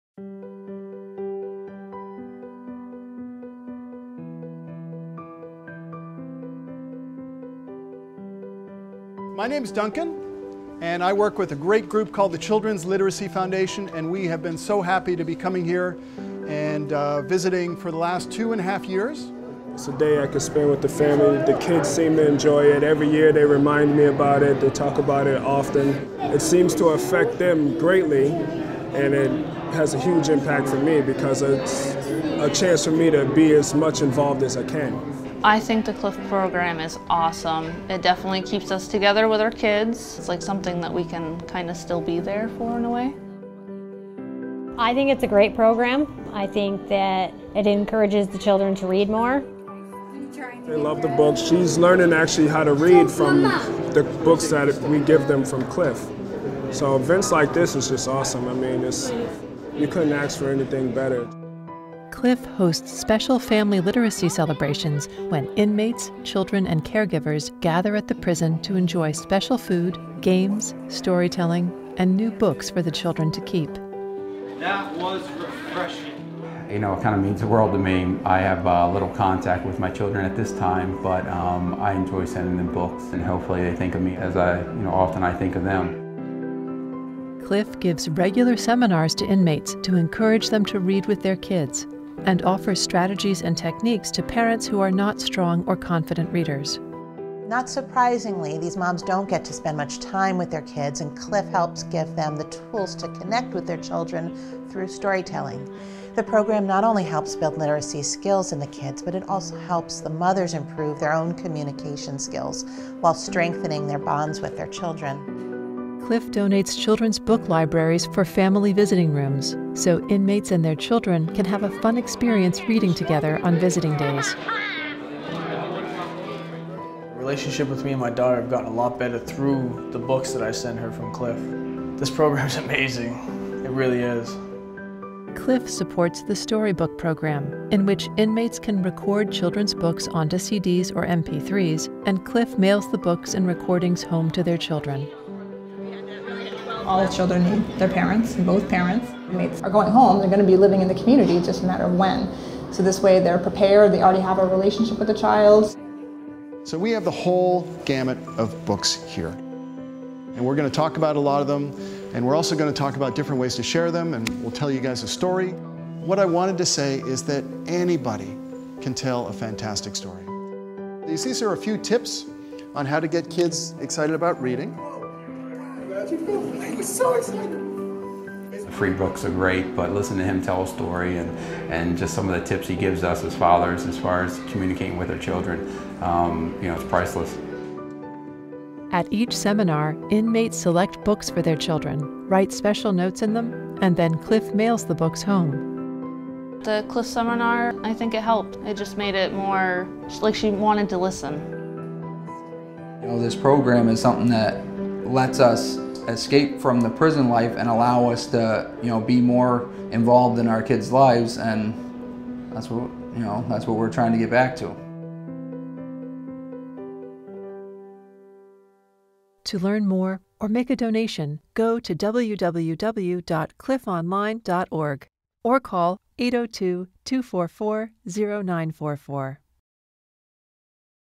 CLiF Children of Incarcerated Adults
The Children's Literacy Foundation (CLiF)'s Children of Incarcerated Adults program serves incarcerated parents and their families in New Hampshire and Vermont.